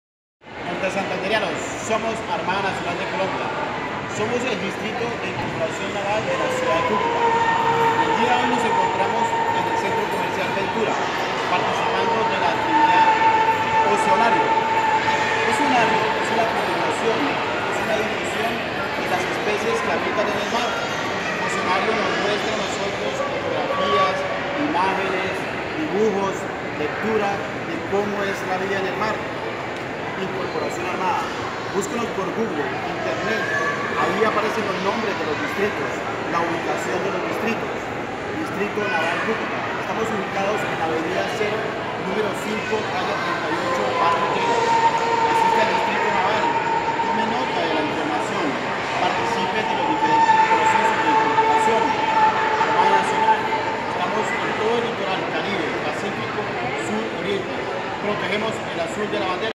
Invitación desde Centro Comercial Ventura en Cúcuta
Invitación a ser parte de la Armada Nacional, a los jóvenes del Departamento a tomar la decisión de ser Marinos de Colombia.